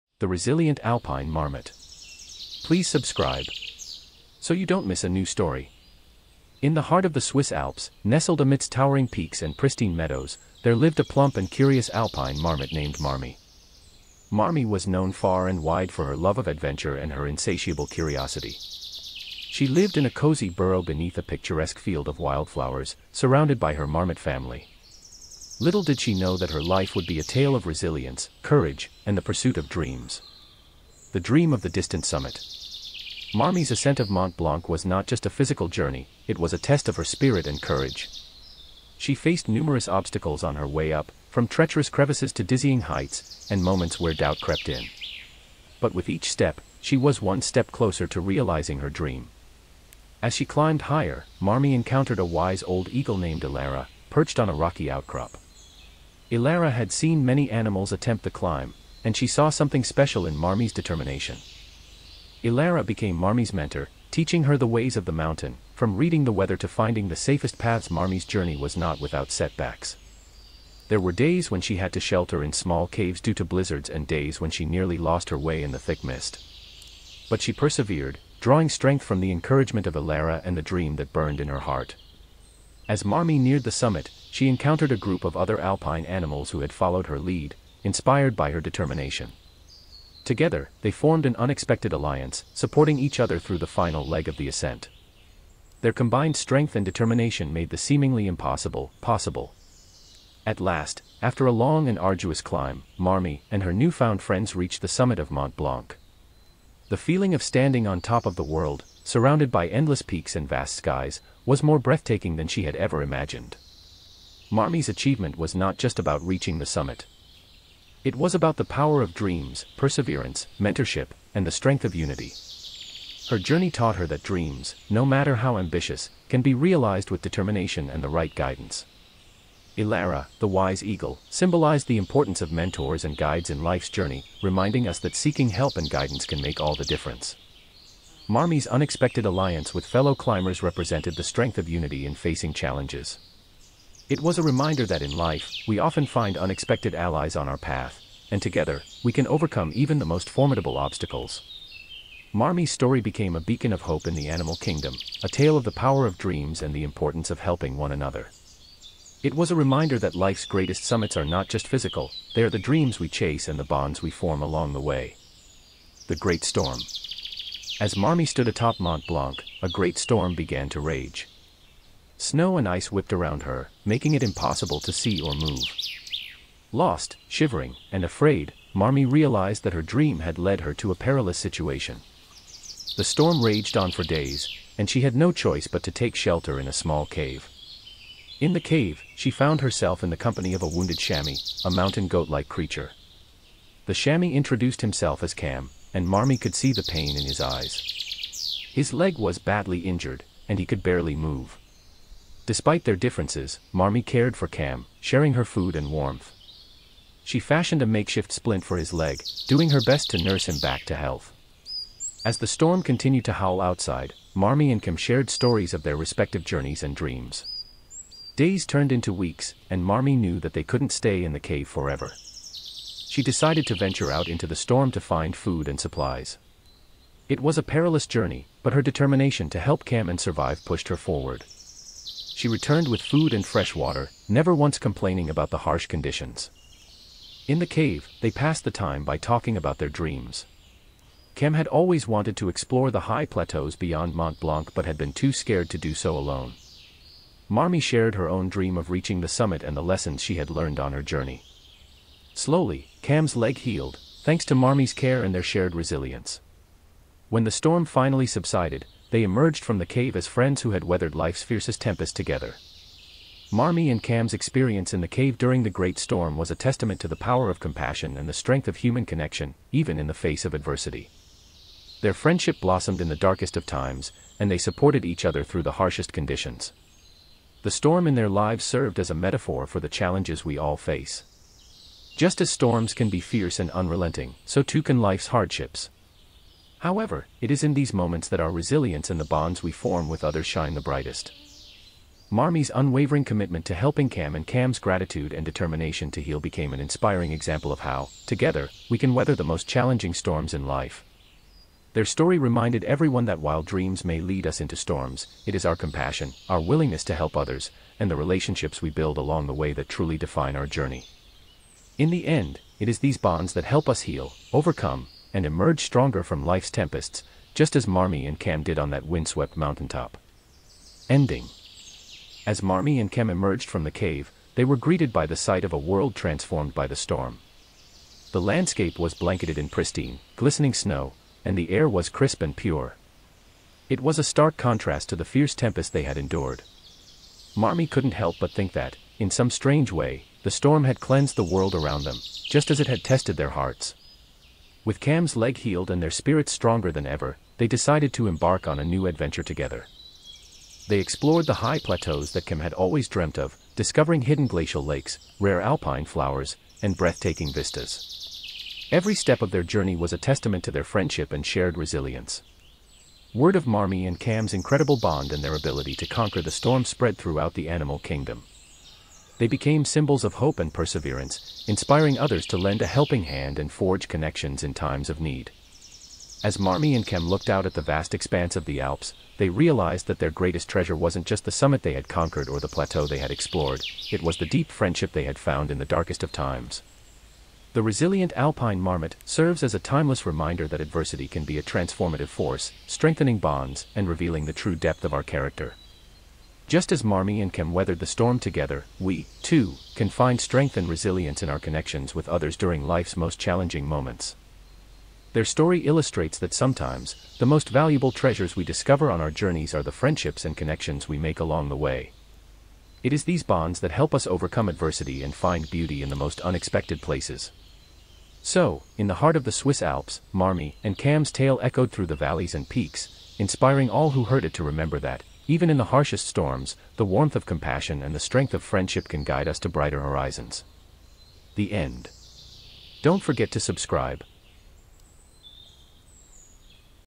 Short Stories 081: The Resilient Alpine Marmot
Introduction: (0:00)
Join us on an extraordinary journey into the majestic Swiss Alps, where the heartwarming tale of "Stormbound Hearts: The Tale of Marmie and Cam" unfolds. This enchanting story weaves together two captivating plot lines that will inspire and uplift your spirits.

The Dream of the Distant Summit: (0:30)

Meet Marmie, the plump and curious alpine marmot with an insatiable thirst for adventure. Follow her as she embarks on a daring quest to conquer the highest peak in the Alps, Mont Blanc. Witness her unwavering determination, the challenges she faces, and the unexpected friendships she forms along the way. This plotline reminds us that dreams are worth pursuing, and with resilience and the right guidance, we can reach the summits of our ambitions.

The Great Storm: (2:45)

Experience the gripping account of Marmie and Cam, a wounded chamois, as they find themselves trapped in a ferocious mountain storm. Their survival against all odds, the deep bond they form in the face of adversity, and their shared journey towards healing is an inspiring metaphor for life's challenges. This story shows us the power of compassion and the strength of human connection, even amidst life's fiercest tempests.

Ending: (5:20)

Discover how Marmie and Cam's story unfolds beyond the storm, as they embark on new adventures, explore breathtaking plateaus, and become symbols of hope and resilience in the animal kingdom. Their tale is a testament to the transformative nature of adversity and the enduring value of friendship.

🌟 Join us in celebrating the resilience of the heart and the beauty of human connections in the midst of life's storms. "Stormbound Hearts: The Tale of Marmie and Cam" is a heartwarming reminder that dreams, compassion, and the bonds we create along our journey are the true treasures of life.

Don't miss this enchanting tale that will leave you inspired and moved. 📽️✨